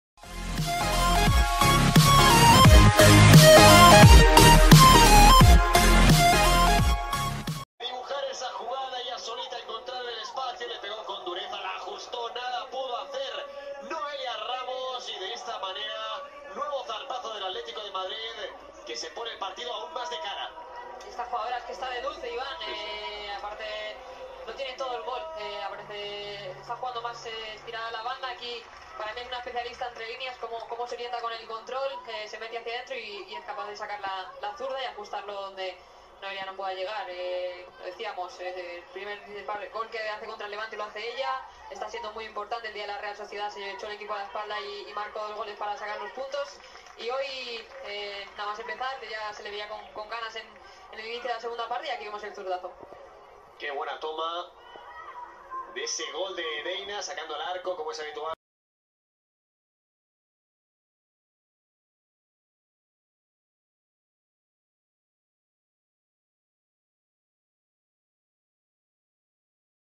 ¡GOLAZO DE DEYNA CASTELLANOS CONTRA EL SEVILLA! ATLÉTICO DE MADRID 3-0 SEVILLA
NADIE PUEDE PARARLA!

Deyna Castellanos suma su octavo gol en la Liga Iberdrola, fue en la victoria del
@AtletiFemenino sobre @SevillaFC con un contudente 3 - 0.

Eegida mejor jugadora del mes de diciembre en la liga española

Hoy marcar su 8vo gol de la temporada Saluden a la Reina
Video: Valentina perdomo